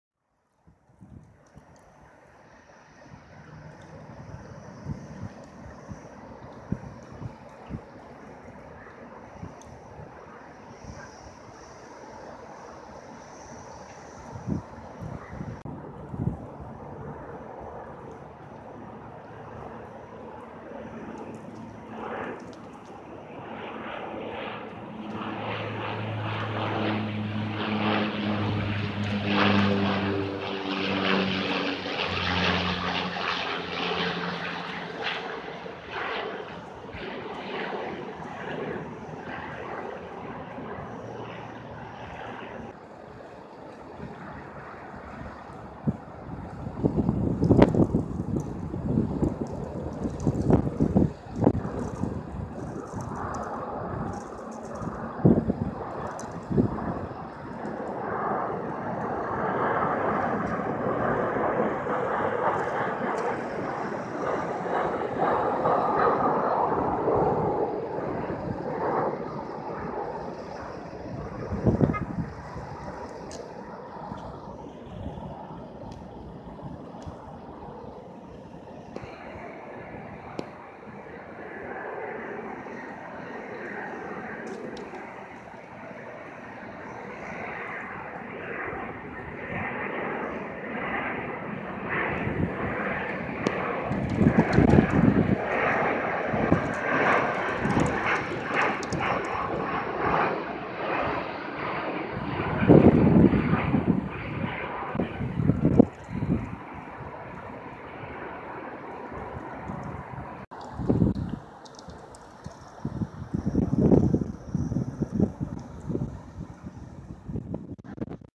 January Afternoon Arrivals to Tampa International Airport
United Airlines, Silver Airways and Frontier Airlines all make brief appearances along with some birds!  I believe the birds are Turkey Vultures.